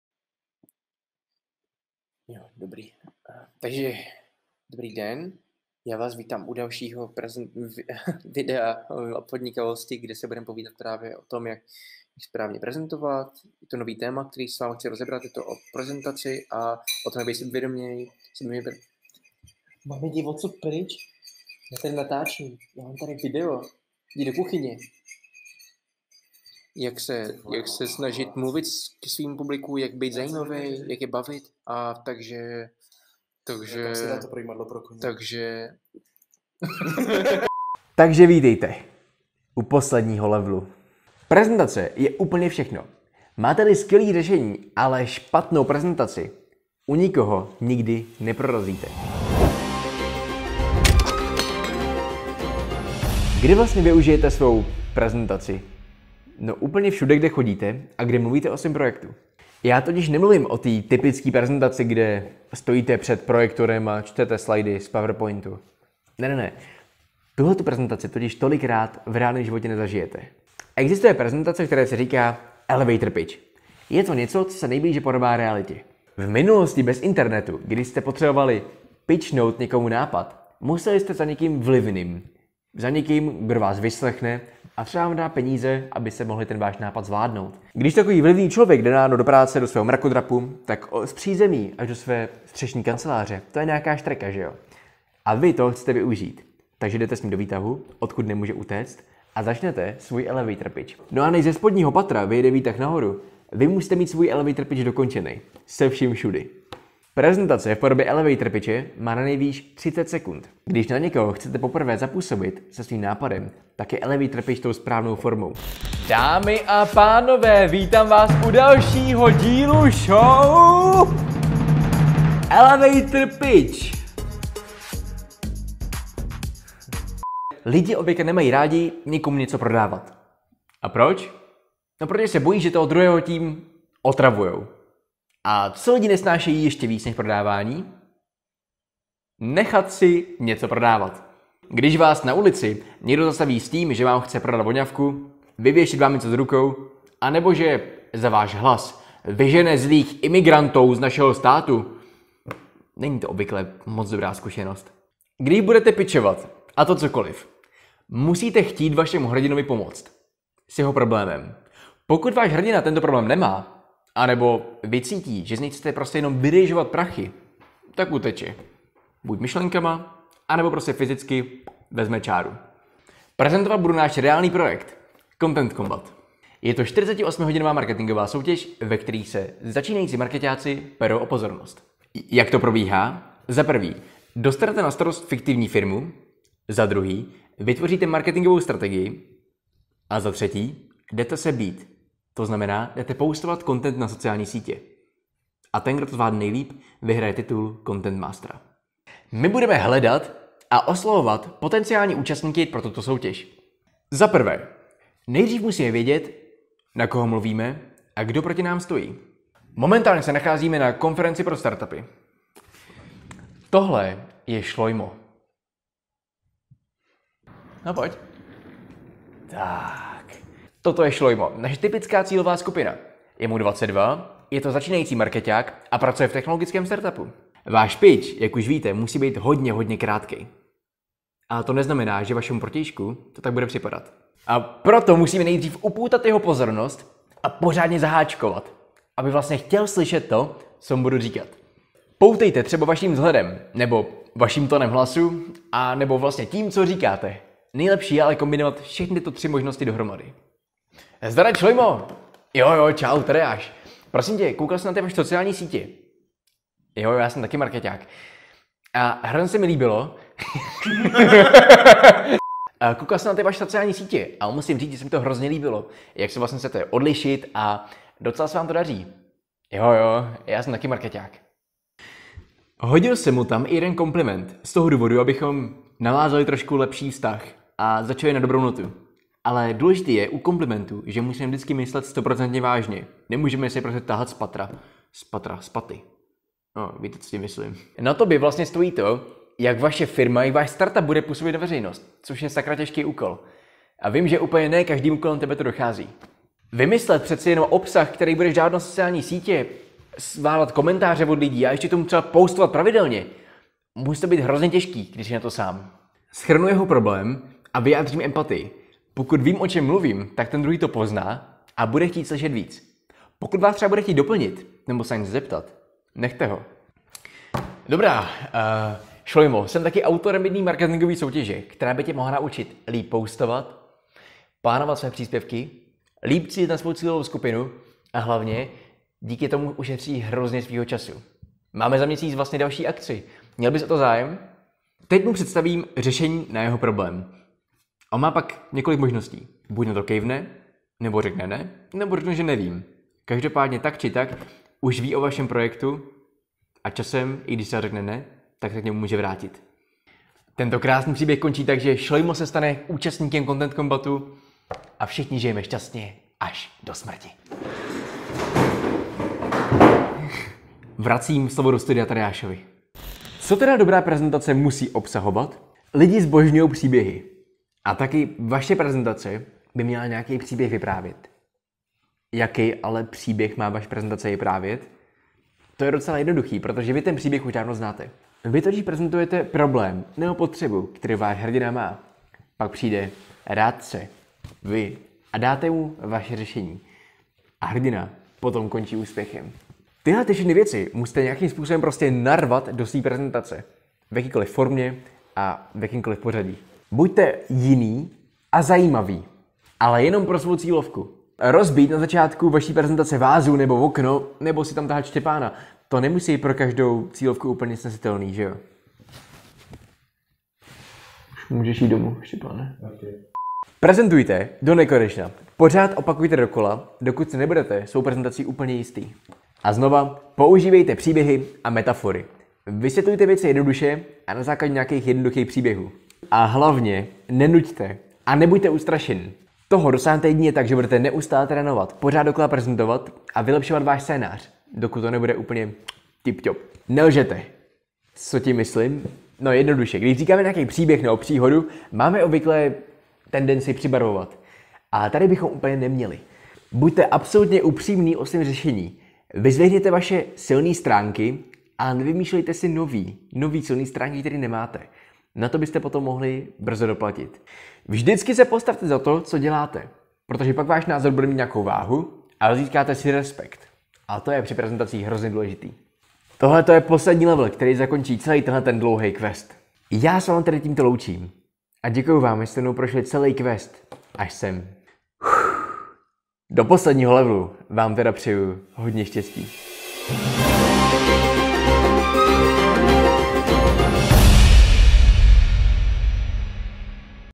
Podnikavost a kreativita - Level 7/7: Velké finále
Cílem spotu je žákům osvětlit možnosti prezentování svých nápadů a jakým způsobem je předat budoucím zákazníkům, investorům apod. Co se žák tedy dozví:
● Prezentace je všechno.
● Komunikace je pilířem podnikavců.
● Umění se prodat není nic špatného.